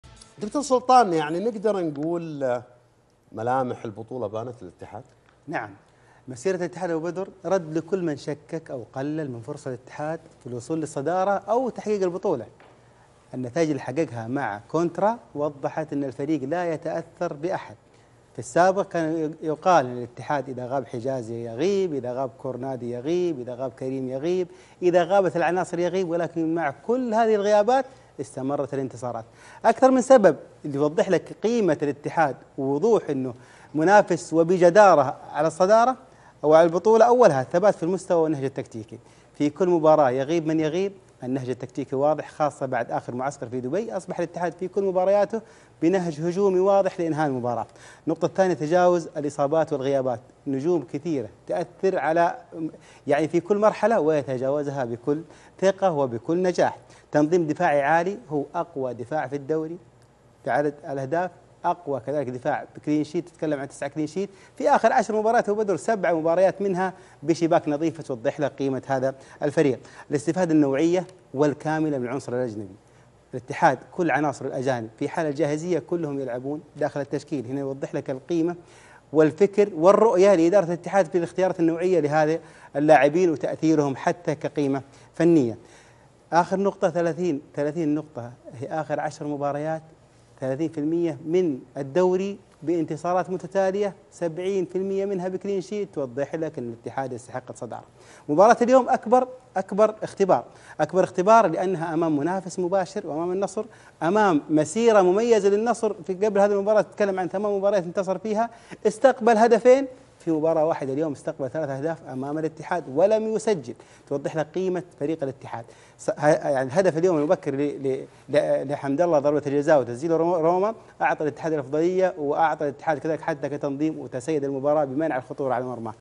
د. سلطان اللحياني: نتائج الاتحاد مع المدرب كونترا تؤكد أن الفريق لا يتأثر بالغيابات بسبب
- ثبات المستوى 👍🏻
- تجاوز ازمة الاصابات🤔
- تنظيم دفاعي 70% بشباك نظيفة 🔥
- الاستفادة من العنصر الأجنبي 😎